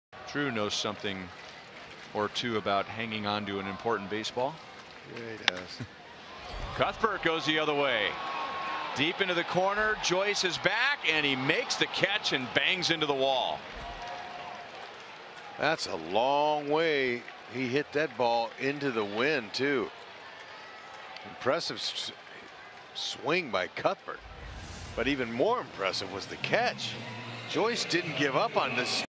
OAK@KC: Matt Joyce collides with the wall on catch
Matt Joyce chases down Cheslor Cuthbert's fly ball to deep right field, and smashes into the wall as he comes away with the grab